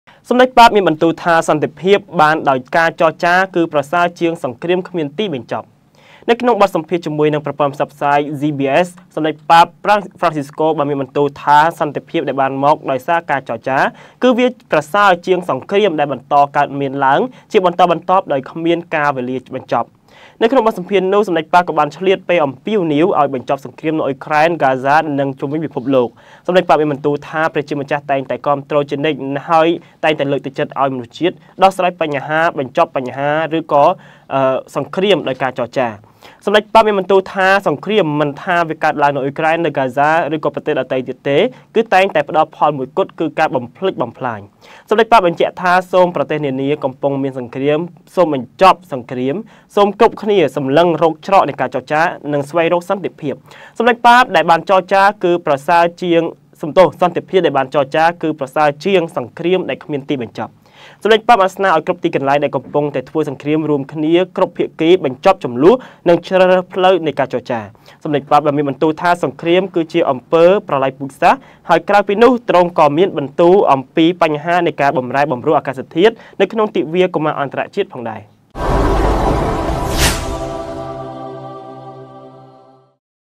សម្តេចប៉ាប៖ សន្តិភាពបានដោយចរចាគឺប្រសើរជាងសង្គ្រាមគ្មានទីបញ្ចប់
នៅក្នុងបទសម្ភាសជាមួយប្រព័ន្ធផ្សព្វផ្សាយ CBS សម្តេចប៉ាប ហ្រ្វង់ស៊ីស្កូបានមានបន្ទូលថា សន្តិភាពដែលបានមកដោយសារតែការចរចាគឺវាប្រសើរជាងសង្រ្គាមដែលបន្តកើតមានឡើង ជាបន្តបន្ទាប់ដោយគ្នាកាលវេលាបញ្ចប់។